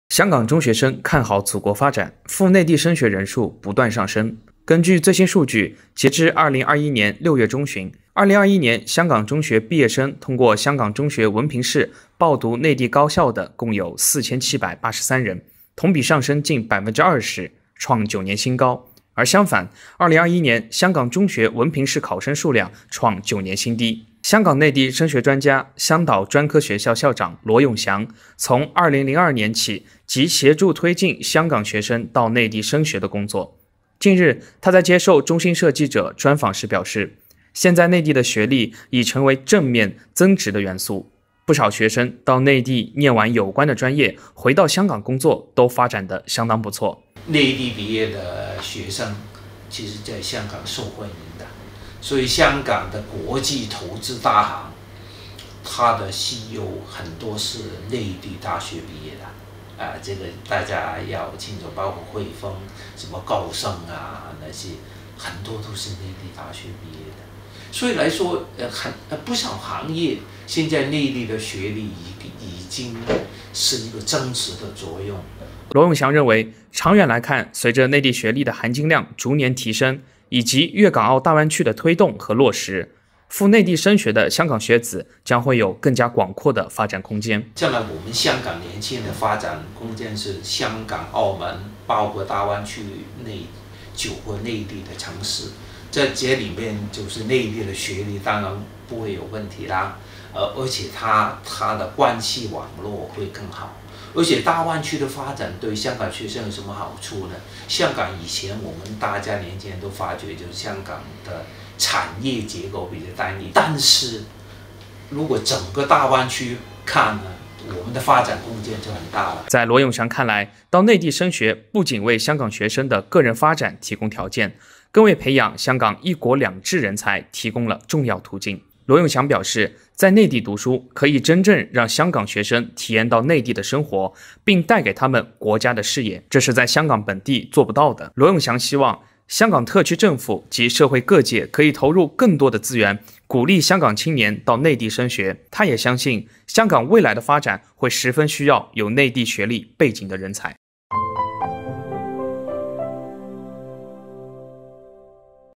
香港内地升学专家罗永祥：内地学习经历已成“增值”元素
香港中学生看好祖国发展，赴内地升学人数不断上升。根据最新数据，截至2021年6月中旬，2021年香港中学毕业生通过香港中学文凭试报读内地高校的共有4783人，同比上升近20%，创九年新高。而相反，2021年香港中学文凭试考生数量创九年新低。
香港内地升学专家、香岛专科学校校长罗永祥从2002年起即协助推进香港学生到内地升学的工作。近日，他在接受中新社记者专访时表示，现在内地的学历，已成为正面、增值的元素，不少学生到内地念完有关的专业，回到香港工作都发展得相当不错。